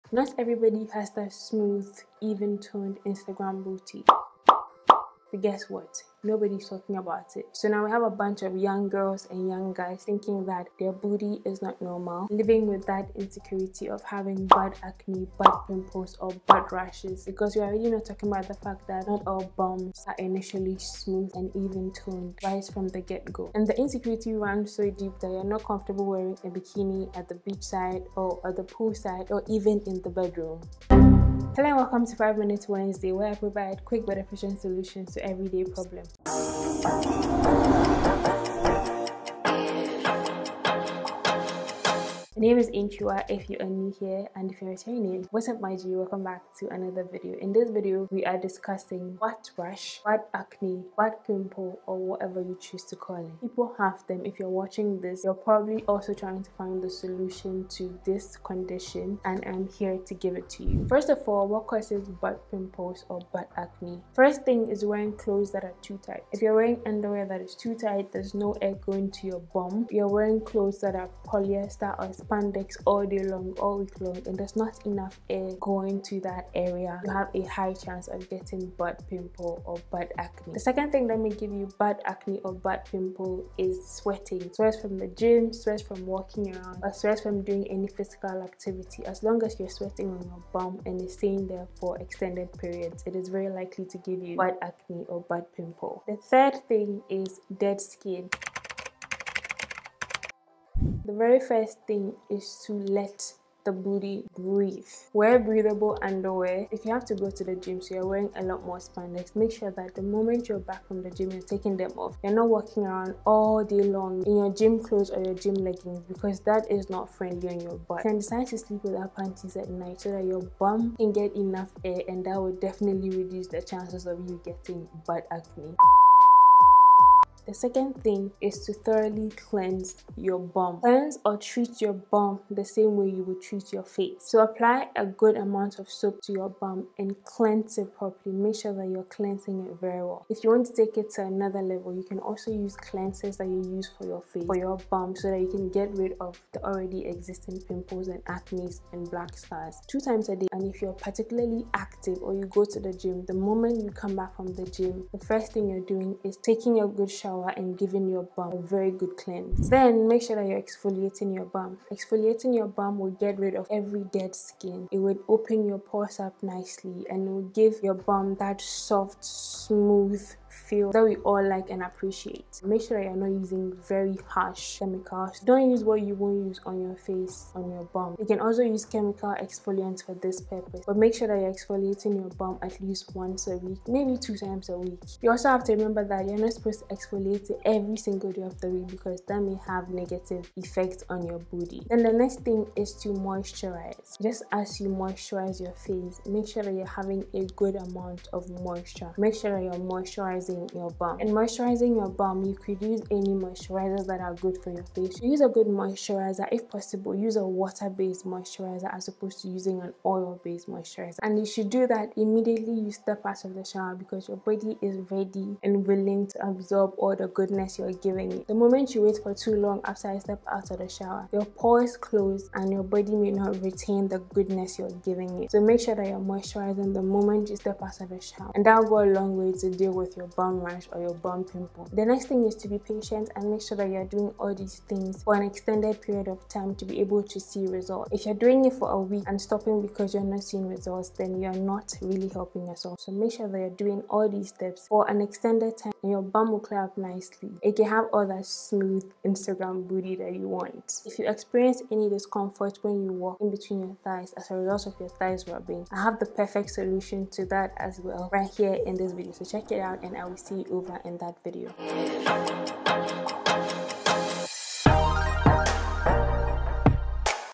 How to get rid of YOUR UGLY BUTT ACNE ( 4 Simple Steps)
How to get rid of butt acne: Butt acne, butt pimples, or whichever way you call it are the absolute worse. The mess with your self-esteem and your classy lady/ big aunty energy.
I believe looking good and feeling good isn't only about the outside but also the inside and what you're hiding under clothes.
I hope these 4 simple steps help you too.